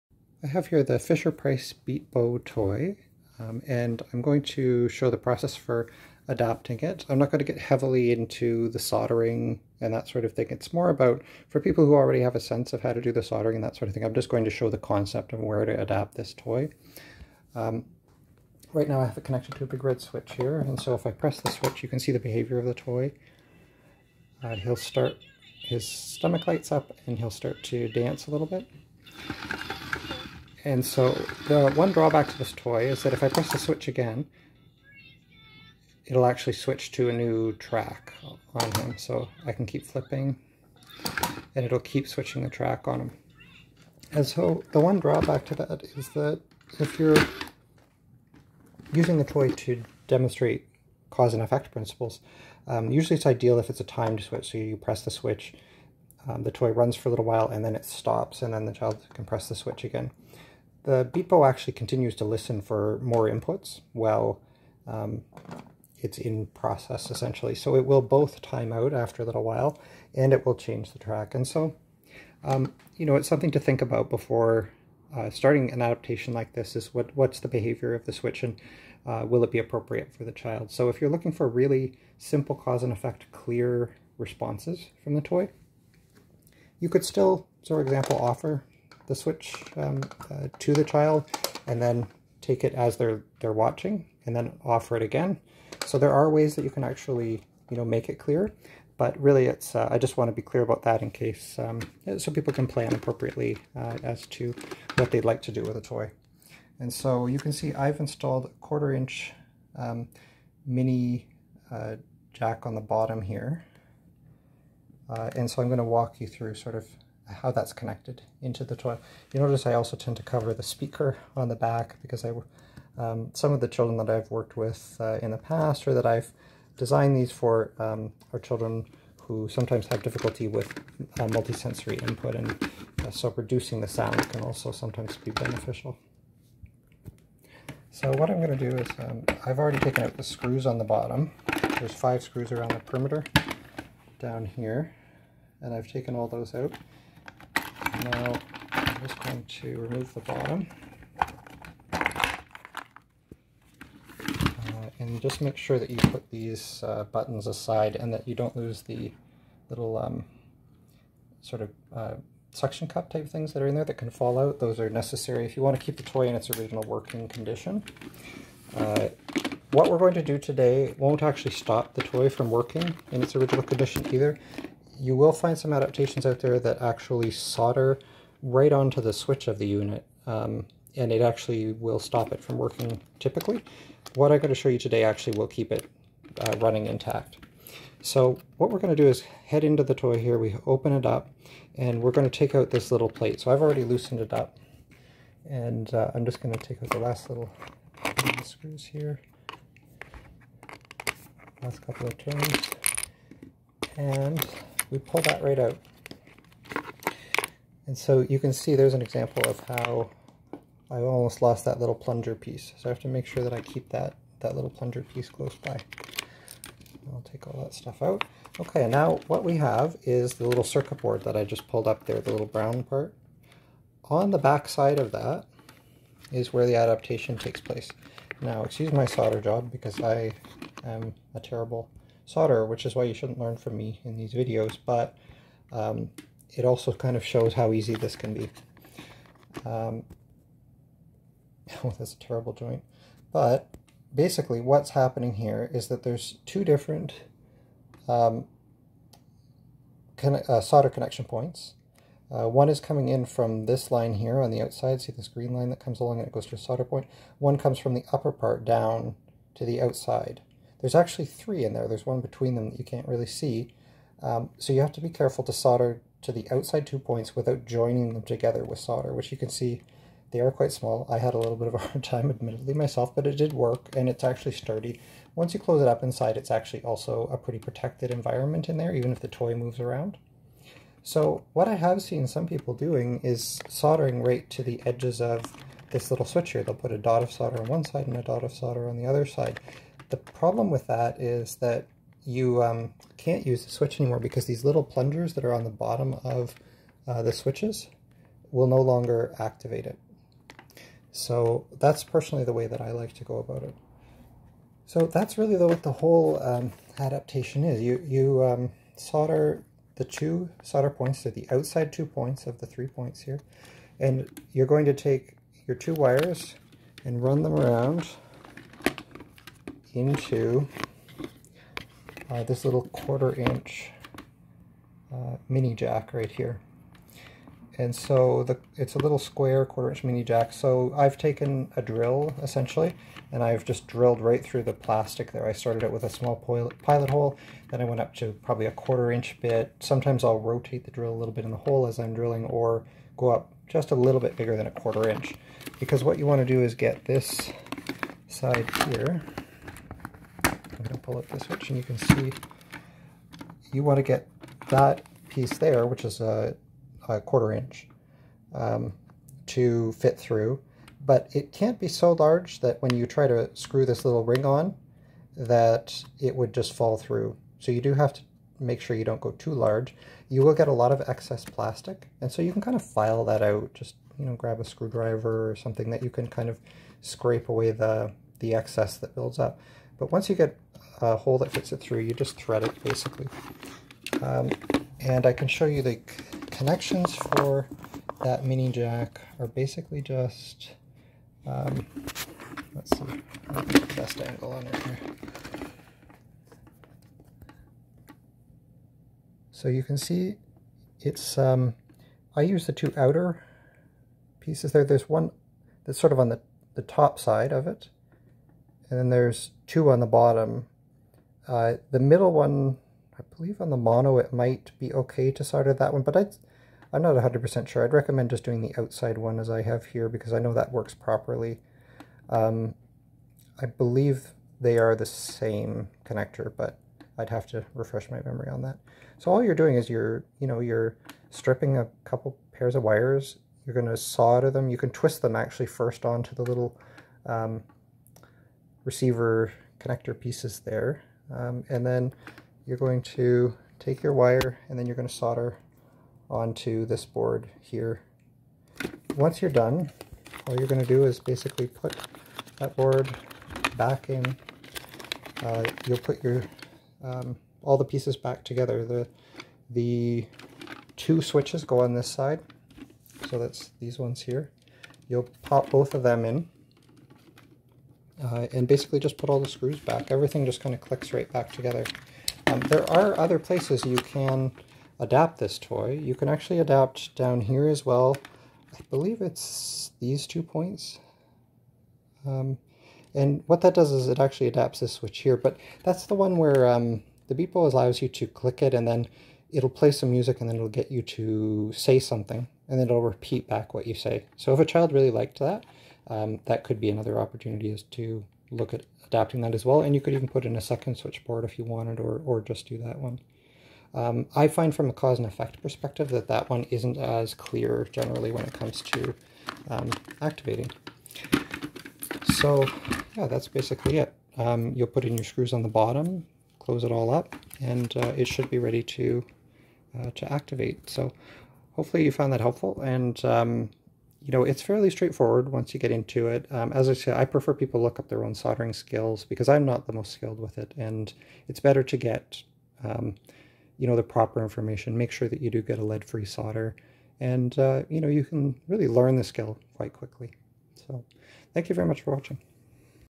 Switch Adapted Beat Bo Toy
How to Adapt the fisher price beat bo toy for use with a single switch.